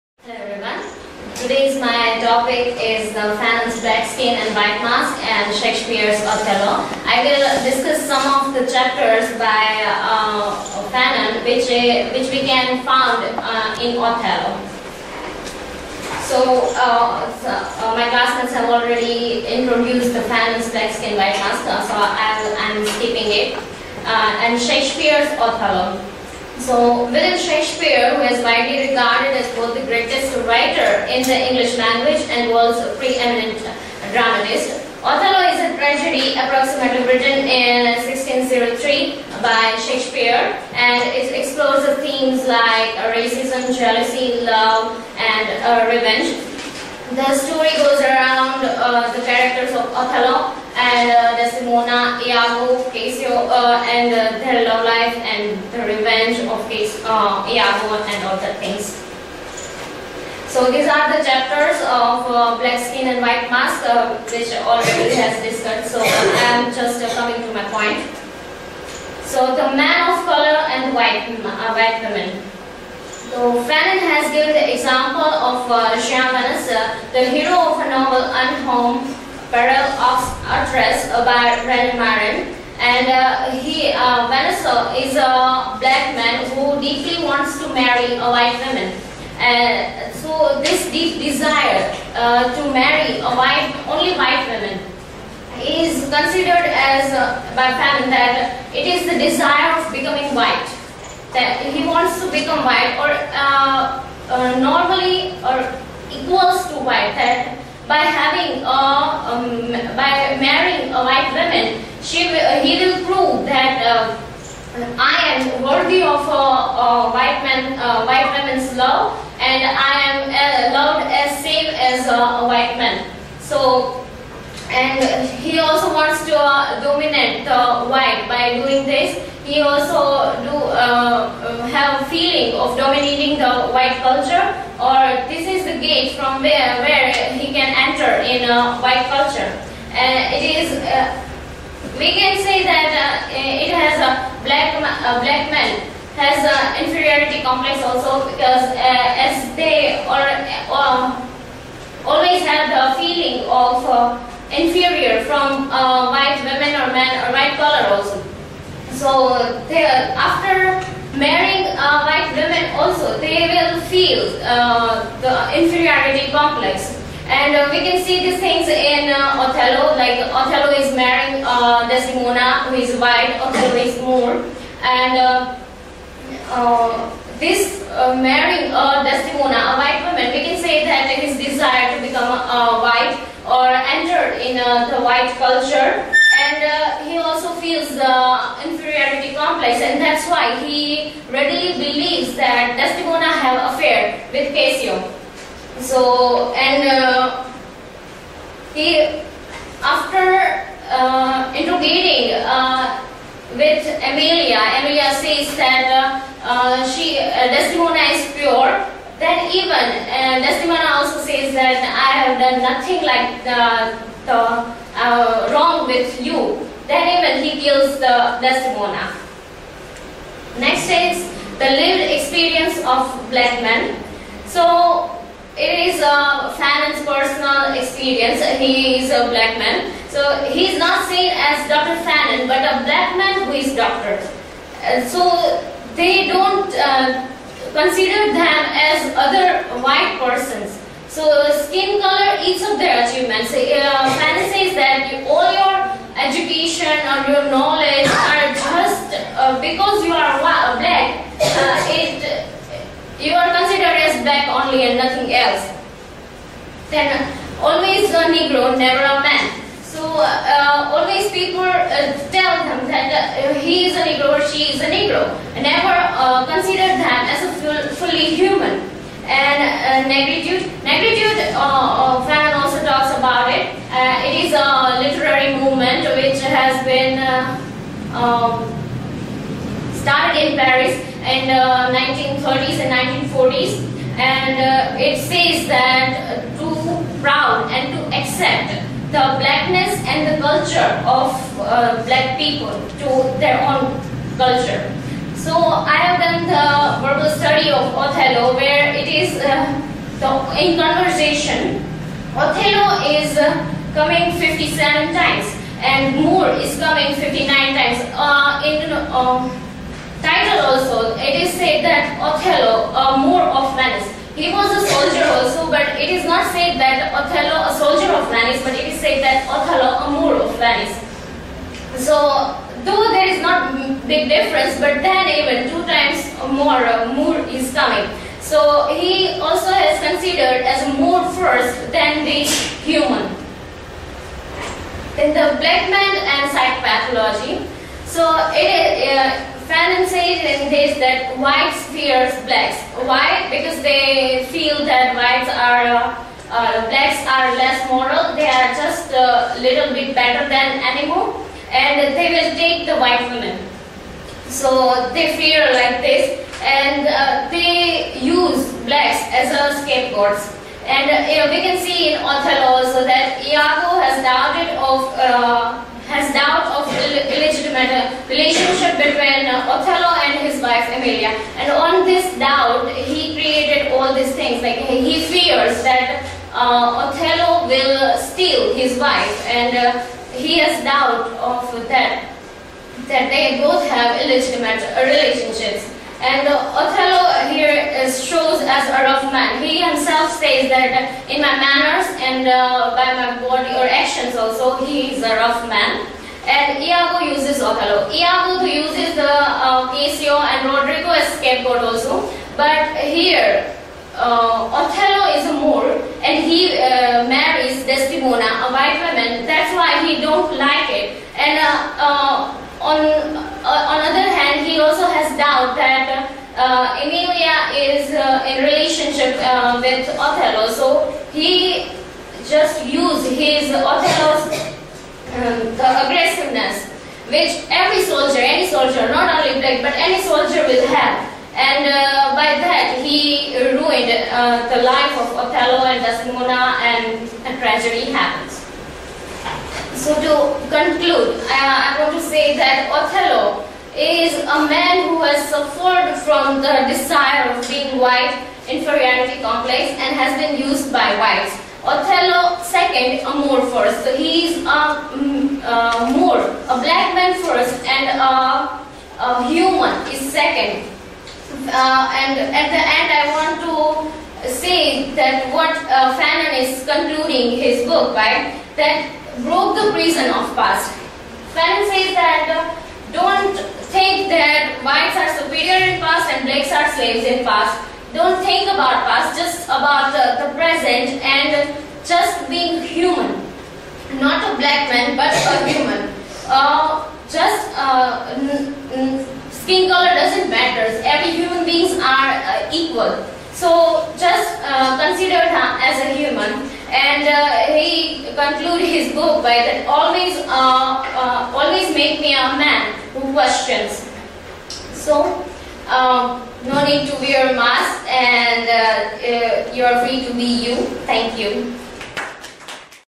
Fanon's "Black skin White masks" and Shakespeare's "Othello"
This video is part of my academic activity. This is presentation which I have presented in class.